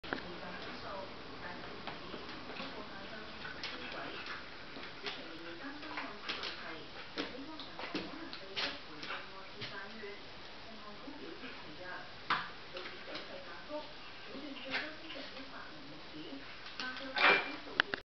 cat vs. dog 3
Labrador (Tequila) plays with Tabby (Gogo)